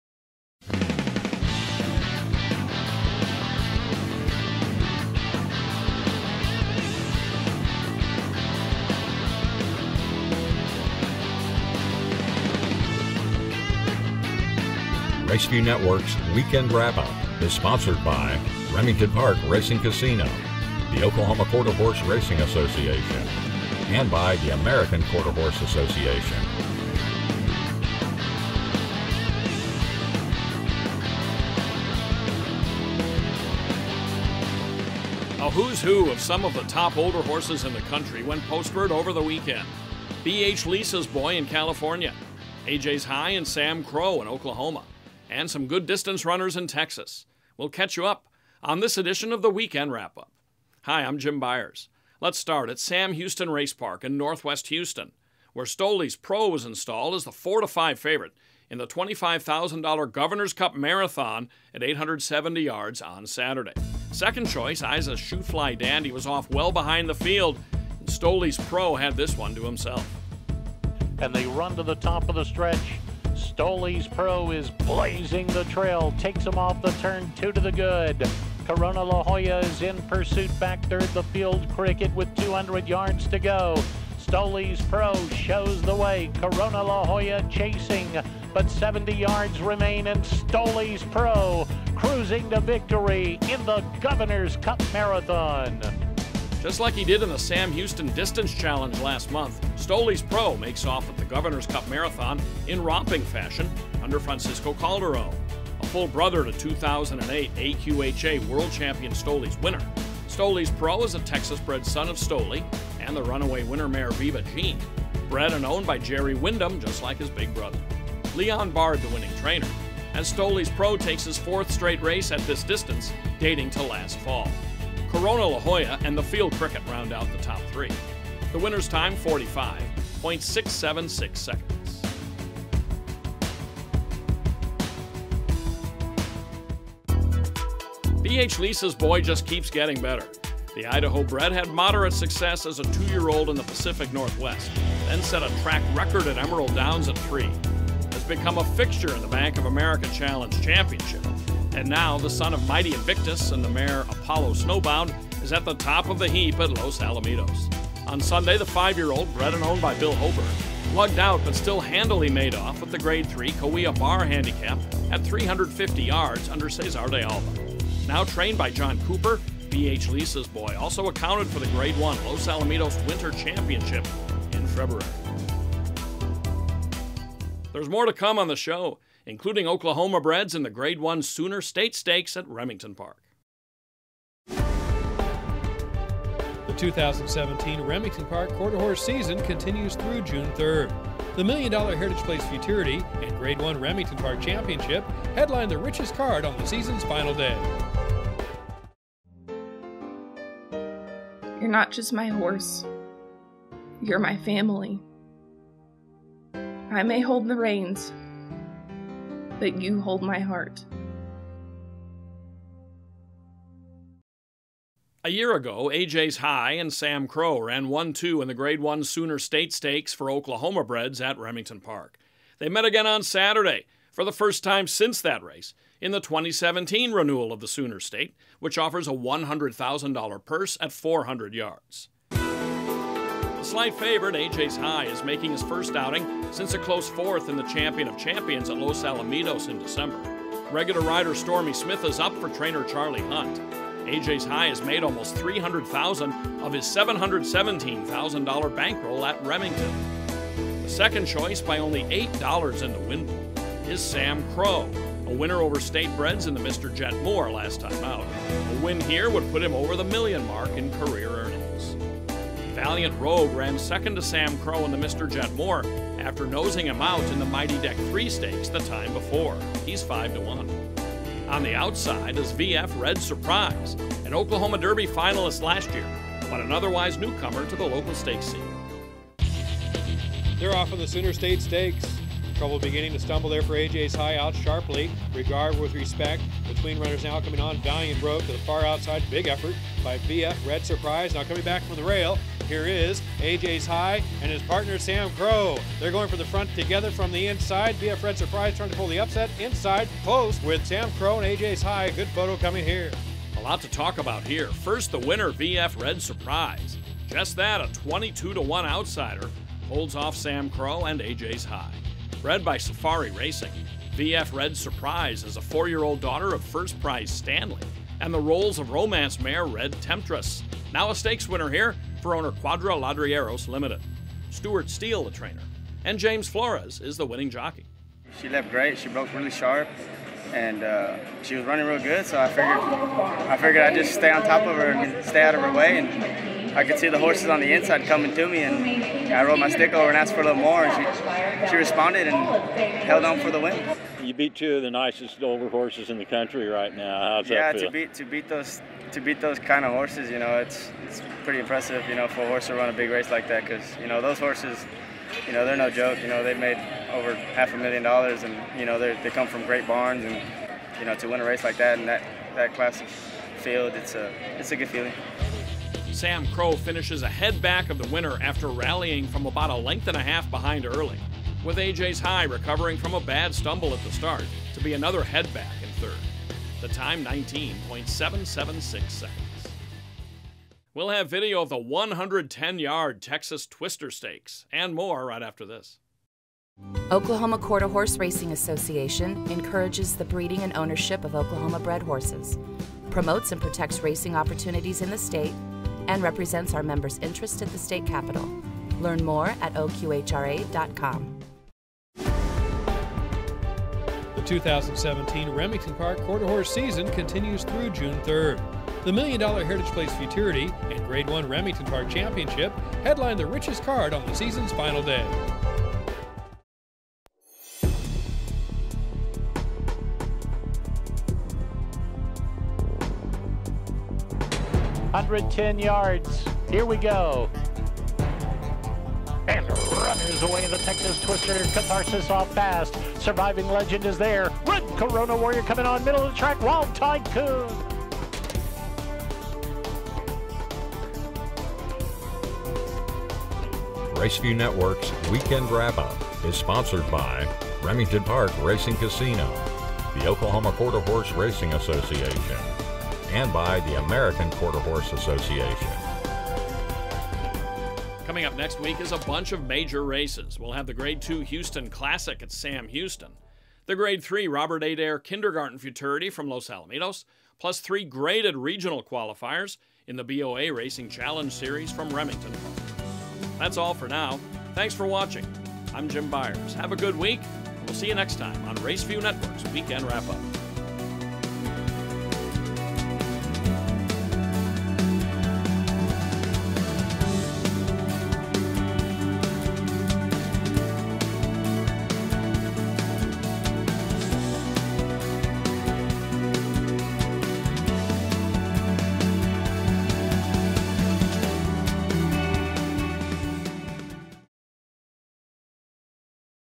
Weekend WrapUp - May 8, 2017
Longshot VF Red Surprise stages big upset in the $100K Sooner State Stakes RG1, while BH Lisas Boy continues his dominance at Los Al.  Plus, Stolis Pro posts second 870-yard stakes win at Sam Houston.  Check out the new Weekend WrapUp for those races and more!